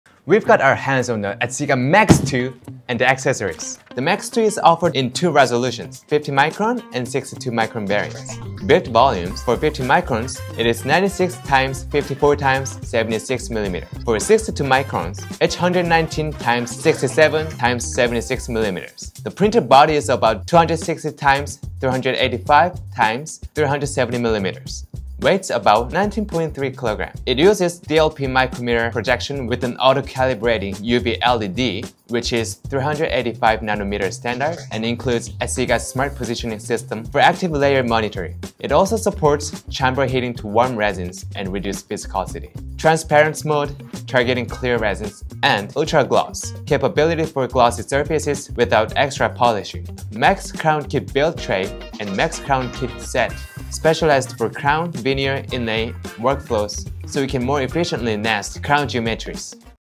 Meet the next generation of dental 3D printing: the Asiga MAX 2 🦷✨
✔️ Two resolutions: 50 µm & 62 µm
✔️ Smart Positioning System for accuracy
✔️ Transparent Mode™ for clear resins
✔️ UltraGLOSS™ for polish-free shine
✔️ Specialized Crown Kit for efficient nesting

All packed into a compact, 19 kg powerhouse built for precision and efficiency.

👉 Are you ready to see what’s possible with the Asiga MAX 2?